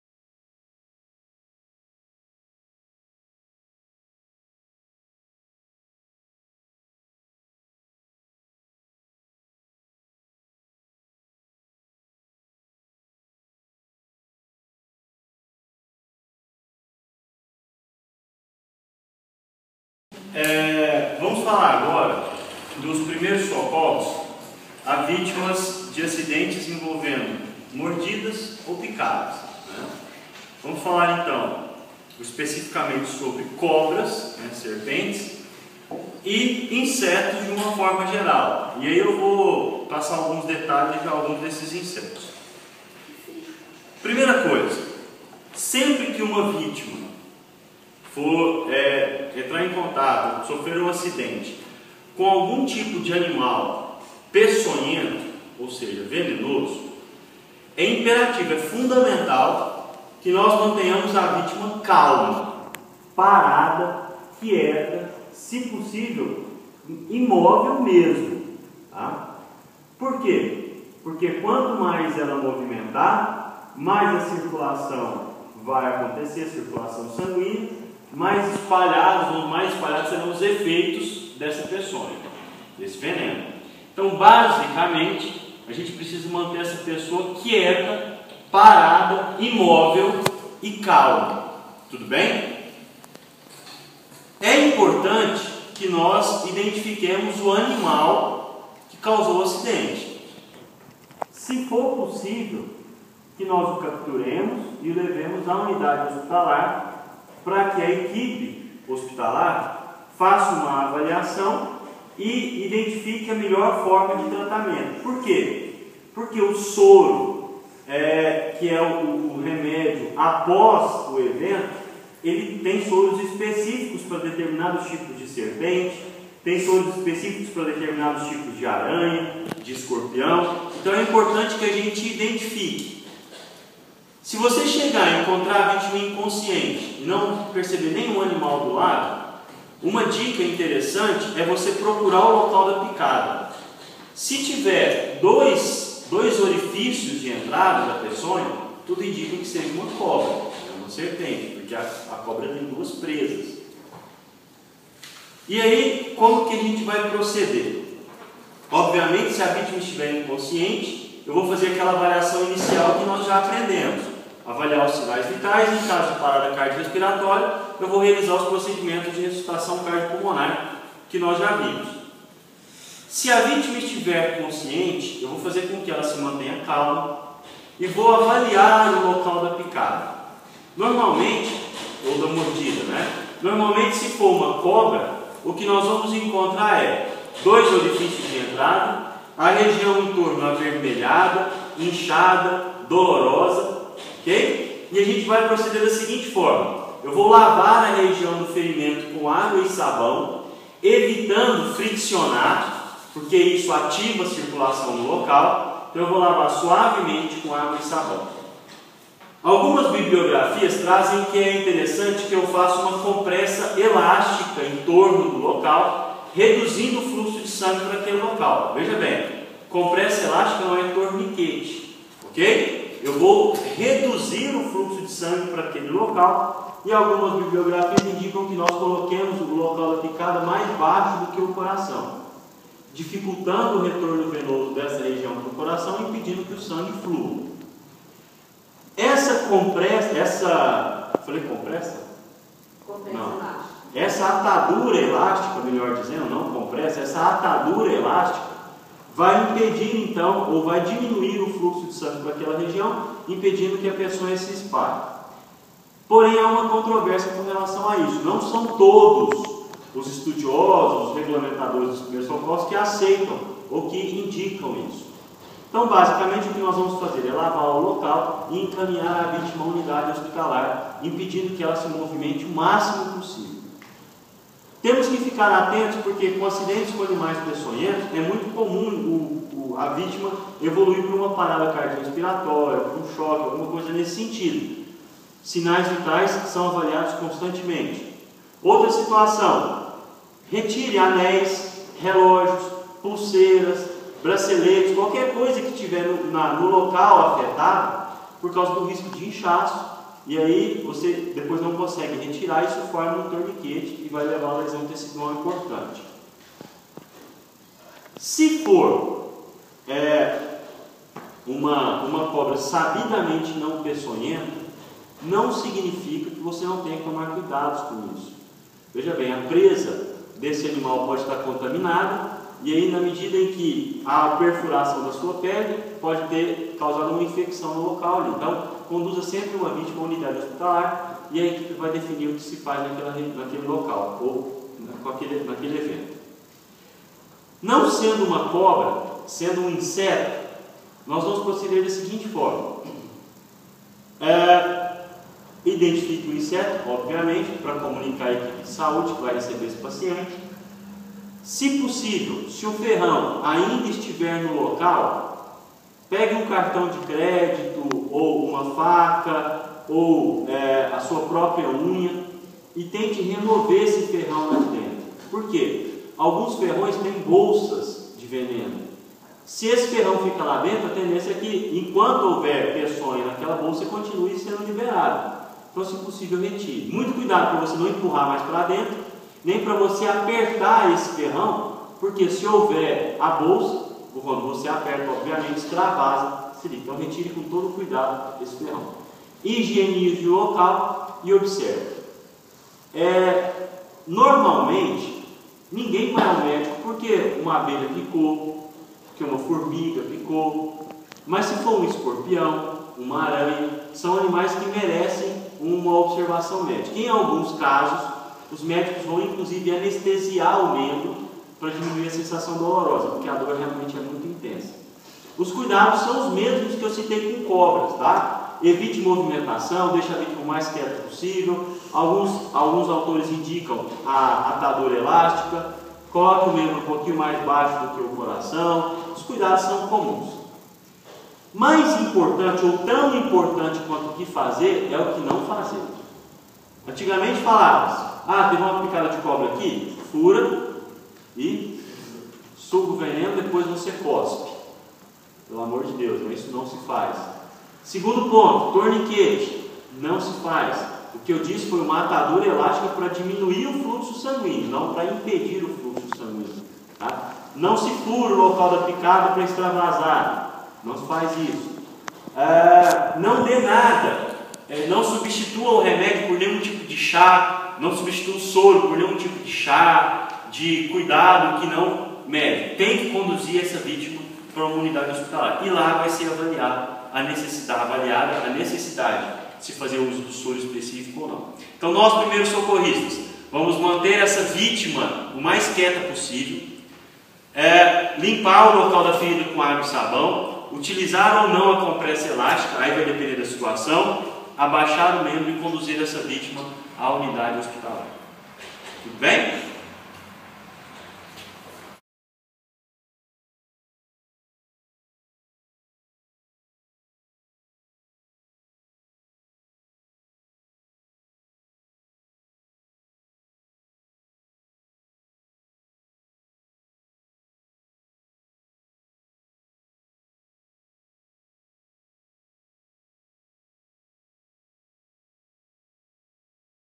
Primeiros Socorros: Mordidas e Picadas de Animais Peçonhentos
Mais uma gravação de minha aula na disciplina de Primeiros Socorros em que apresento as medidas básicas a serem tomadas pelo primeiro socorrista nos acidentes que envolvam animais peçonhentos.